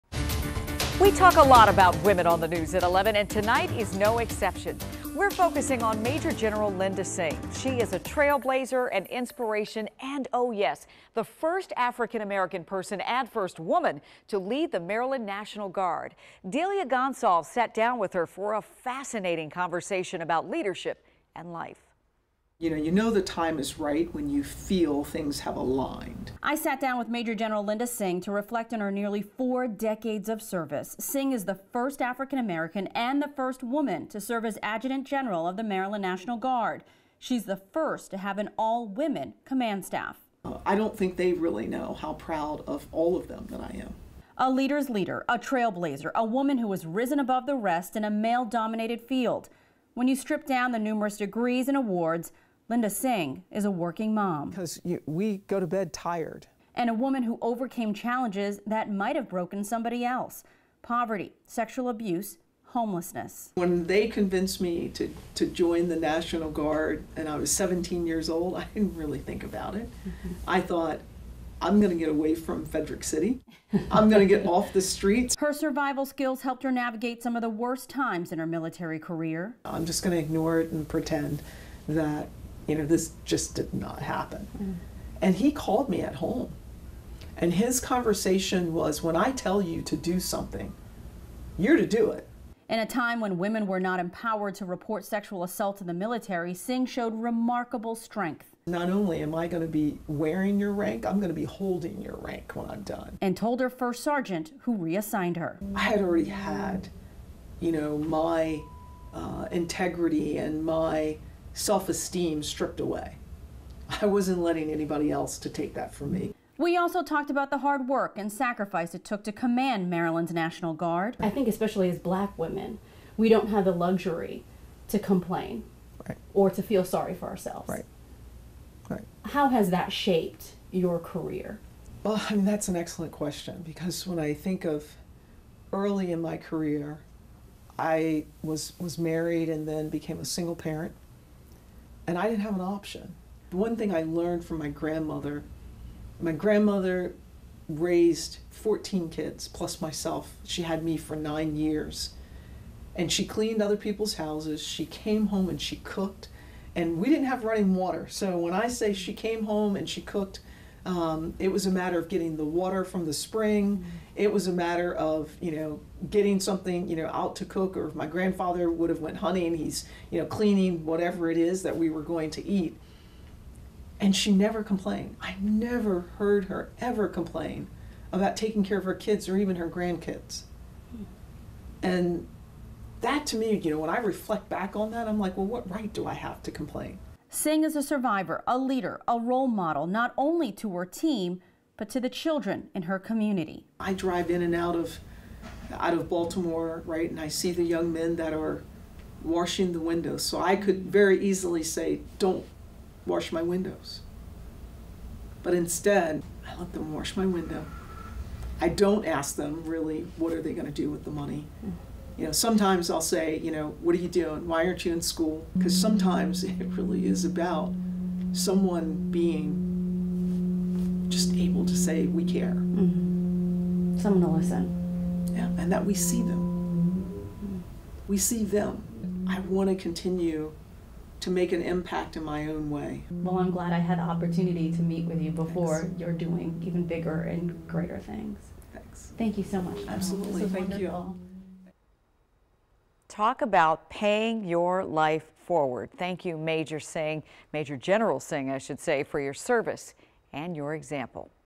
Meet the African-American and woman to lead Md. National Guard
She's been serving for nearly four decades. She's the first African-American and the first woman to serve as adjutant general of the Maryland National Guard. "You know the time is right when you feel things have aligned," Major General Linda Singh said.